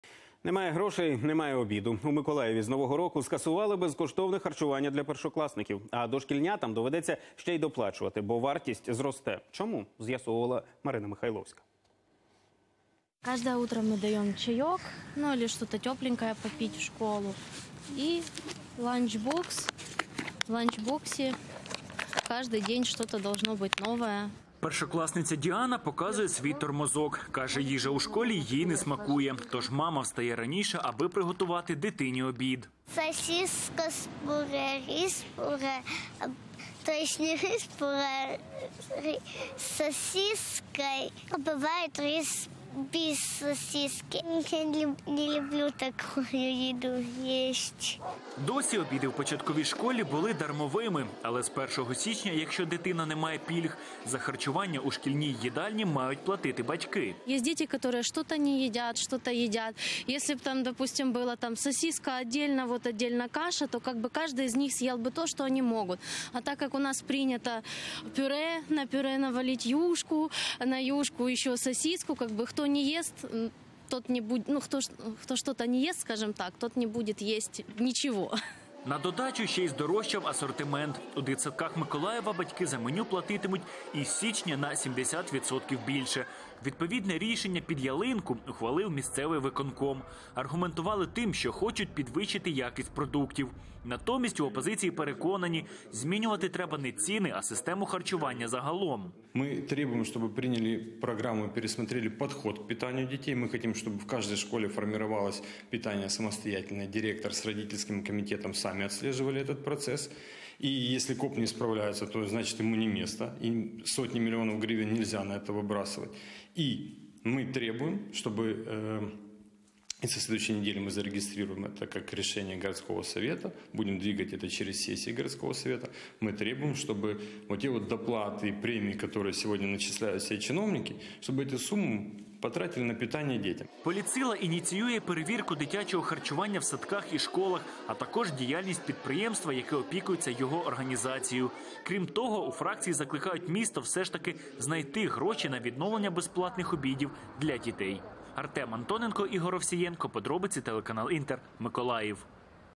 "Опозиційна платформа - За життя" вимагає відновити безкоштовне харчування школярів у Миколаєві
Політсила ініціює перевірку дитячого харчування у дитсадках і школах, а також діяльності підприємства, яке опікується його організацією